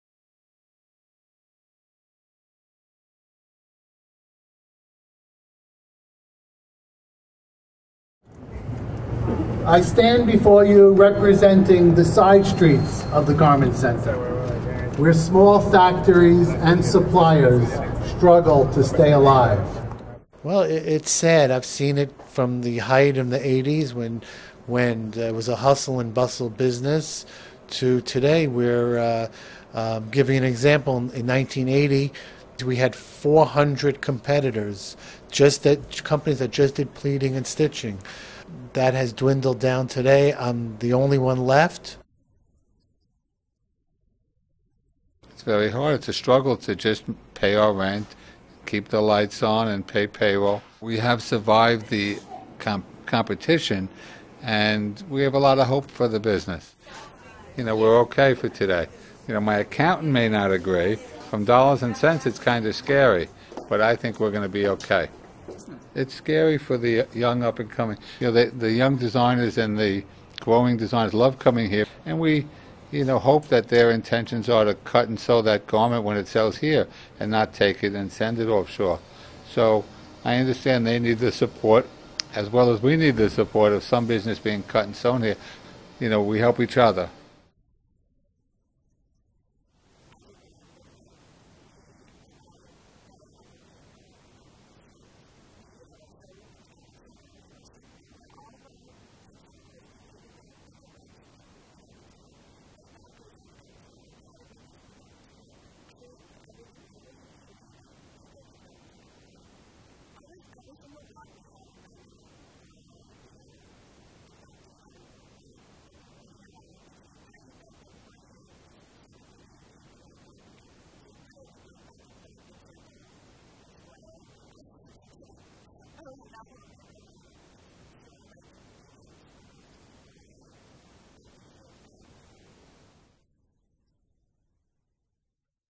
Voices from the Garment District
Walking down 7th Avenue, it is hard to miss the giant needle threading a button at the corner of the 39th Street. A few steps away you will see a life-sized bronze statue of an old man, hunched over a sewing machine and wearing a yarmulke. These two landmarks are the most visible reminders of what used to be one of America's largest manufacturing hubs: The Garment District, a less than one square mile area in Manhattan that used to be the source for most clothes worn in the United States until the middle of the last century...

Video produced by Ale Borunda and Ahmed Al Omran